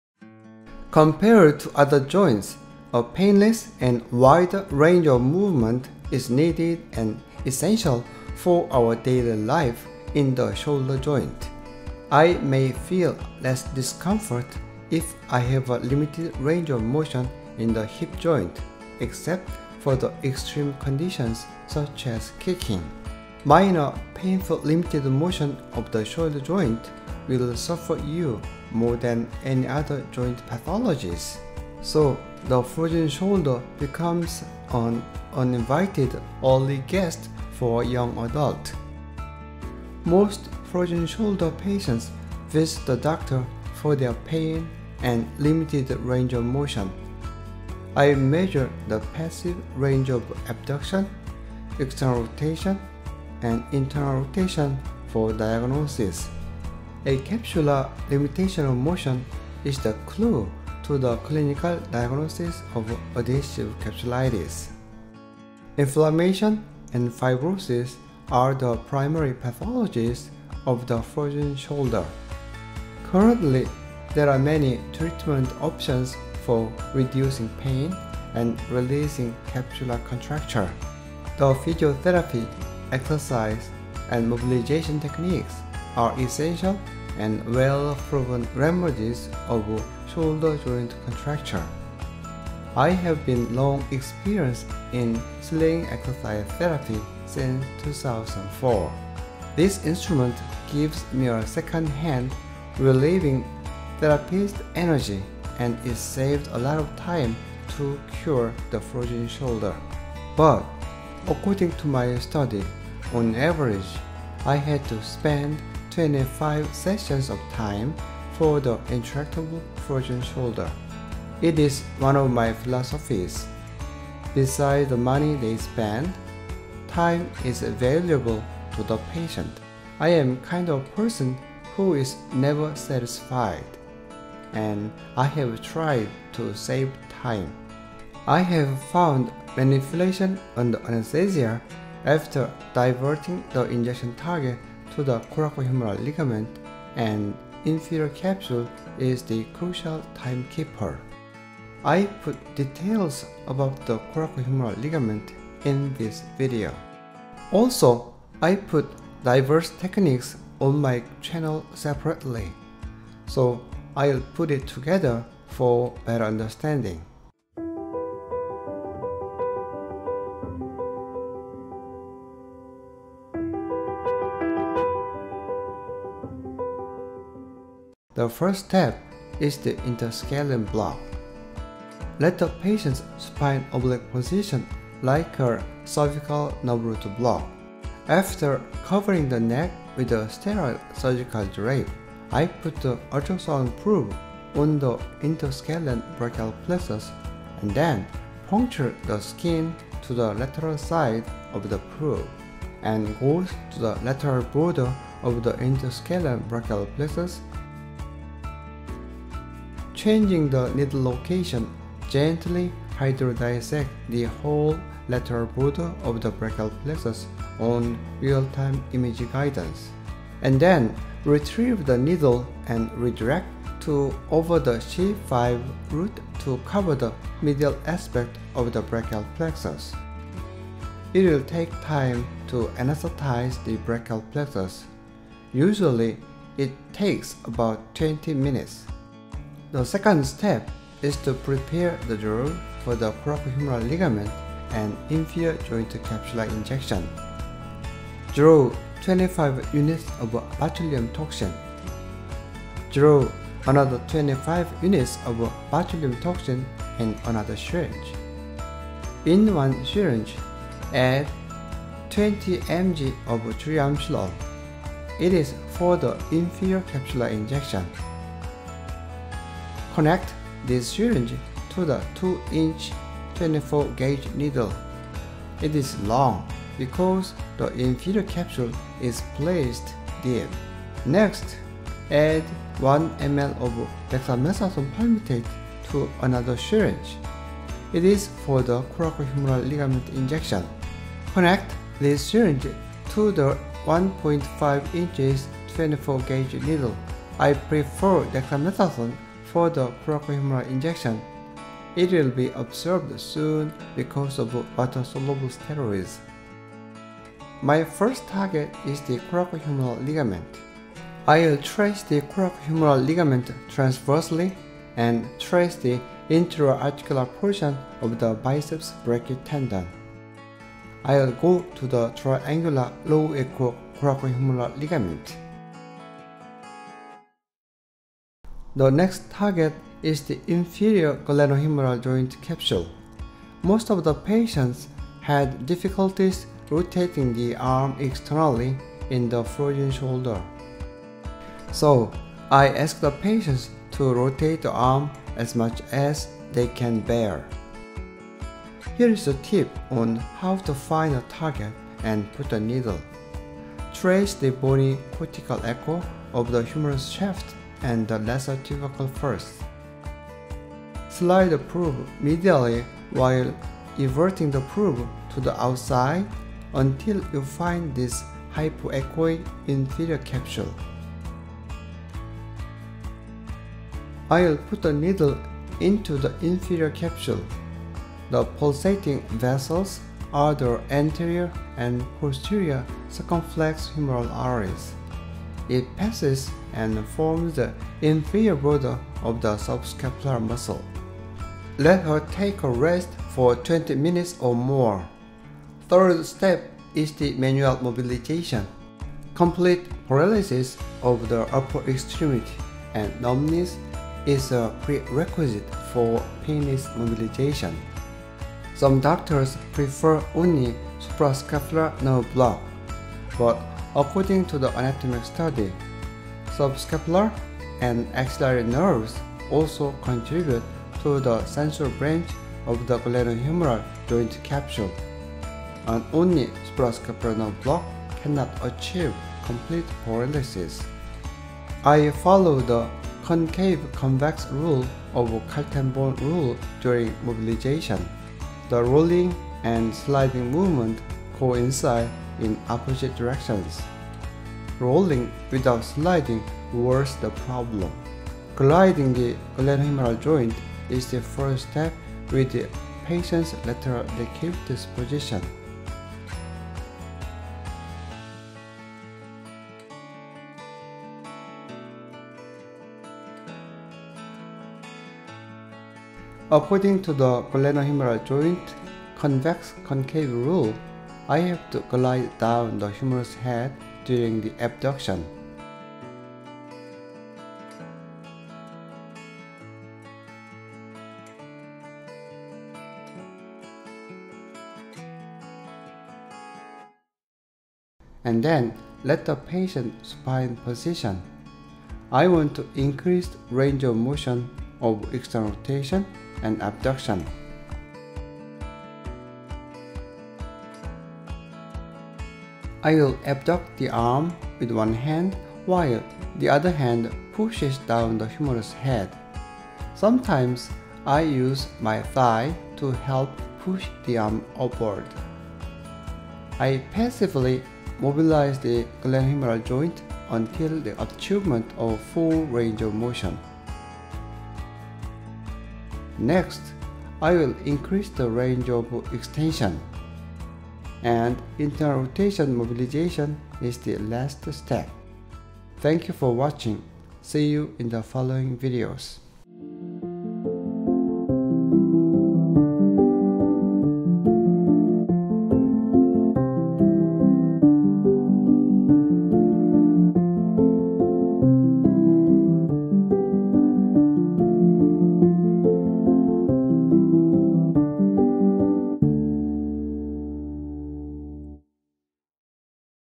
Comprehensive ultrasound guided interventions for the intractable frozen shoulder
Compared to other joints, a painless and wider range of movement is needed
and essential for our daily life in the shoulder joint.
I may feel less discomfort if I have a limited range of motion in the hip joint
except for extreme conditions such as kicking.
Minor painful limited motion of the shoulder joint will suffer you
more than any other joint pathologies.
So, the frozen shoulder becomes an uninvited early guest for a young adult.

Most frozen shoulder patients visit a doctor for their pain and limited range of motion.

 I measure the passive range of abduction, external rotation, and internal rotation for diagnosis.

A capsular limitation of motion is the clue to the clinical diagnosis of adhesive capsulitis.
Inflammation and fibrosis are the primary pathologies of the frozen shoulder.
Currently, there are many treatment options for reducing pain and releasing capsular contracture.
The physiotherapy, exercise, and mobilization techniques are essential
and well-proven remedies of shoulder joint contracture.
I have been long experienced in sling exercise therapy since 2004.
This instrument gives me a second-hand relieving therapist’s energy,
and it saved a lot of time to cure the frozen shoulder.
But according to my study.
On average, I had to spend 25 sessions of time for the intractable frozen shoulder.
It is one of my philosophies:
Besides the money they spend, time is valuable to the patient.
I am the kind of person who is never satisfied.
And I have tried to save time.
I have found manipulation under anesthesia
after diverting the injection target to the coracohumeral ligament,
 and the inferior capsule is the crucial timekeeper.
I put details about the coracohumeral ligament in this video.
Also, I put diverse techniques on my channel separately.
 So, I will put it together for better understanding.
The first step is the interscalene block.
Let the patient supine oblique position like a cervical nerve root block.
after covering the neck with a sterile surgical drape,
I put the ultrasound probe on the interscalene brachial plexus
 and then punctured the skin to the lateral side of the probe.
and goes to the lateral border of the interscalene brachial plexus .
changing the needle location, gently hydro-dissect
the whole lateral border of the brachial plexus on real-time image guidance.
and then retrieve the needle and re-direct to over the
C5 root to cover the medial aspect of the brachial plexus.
It will take time to anesthetize the brachial plexus. Usually, it takes about 20 min.
The second step is to prepare the drug for the coracohumeral ligament
and inferior joint capsular injection.
Draw 25 units of botulinum toxin.
Draw another 25 units of botulinum toxin in another syringe.
In one syringe, add 20mg of triamcinolone. It is for the inferior capsular injection.
Connect this syringe to the 2-inch 24- gauge needle.
It is long because the inferior capsule is placed deep.
Next, add 1 ml of dexamethasone palmitate to another syringe.
 It is for the coracohumeral ligament injection
connect this syringe to the 1.5 inches 24- gauge needle.
I prefer dexamethasone for the coracohumeral injection.
It will be absorbed soon because of water-soluble steroids.
My first target is the coracohumeral ligament.
I will trace the coracohumeral ligament transversely
and trace the intra-articular portion of the biceps brachii tendon.
I will go to the triangular low echo coracohumeral ligament.
The next target is the inferior glenohumeral joint capsule.
Most of the patients had difficulties rotating the arm externally in the frozen shoulder.
 So, I ask the patient to rotate the arm as much as they can bear.
Here is the tip on how to find the target and put the needle.
Trace the bony cortical echo of the humerus shaft and the lesser tubercle first.
Slide the probe medially while everting the probe to the outer side
 until you find this hypoechoic inferior capsule.
I will put the needle into the inferior capsule.
The pulsating vessels are anterior or posterior circumflex humeral arteries.
 It passes and forms the inferior border of the subscapularis muscle.
Let her take a rest for 20 minutes or more.
Third step is the manual mobilization.